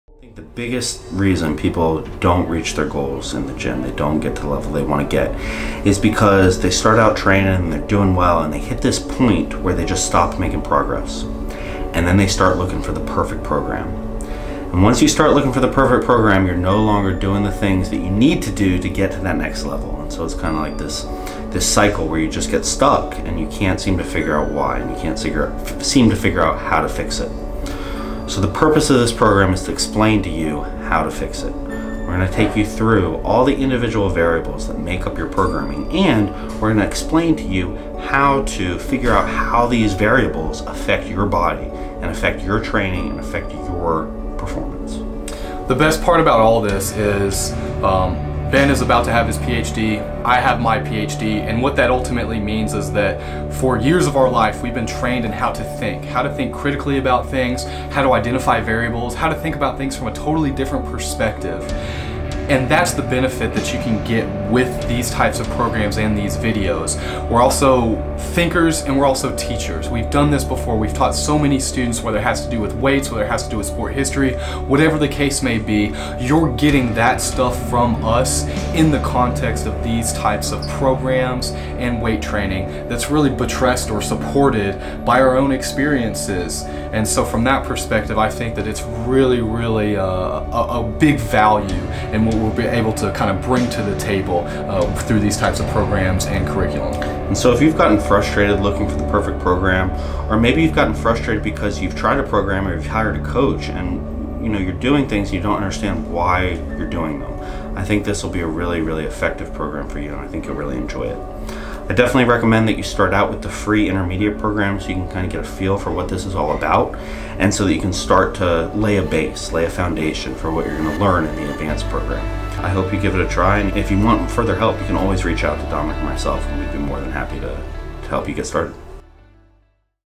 The NEW Unf*ck Your Program
I'm probably more proud of this new program than anything I've ever put out. It was a big project putting all this together:

-5 modules, extending the original Unf*ck series on programming, and adding nutrition, cardio and recovery

- 28 weeks of training and 4 different programs, emphasizing different training variables (like volume, intensity, and frequency)

- The exact water-cut protocol I use for myself and my clients

- Suggestions for implementing RPE-based autoregulation into the plans using my RI Protocols

And, each week of 2018, I'll be posting a new Unf*ck video on this channel about the topics you're most interested in, starting with hypertrophy training.  The first one drops this Friday, so be sure to sub for updates!

And huge, huge thanks to GoodyBEATS for the awesome videography in the advanced course!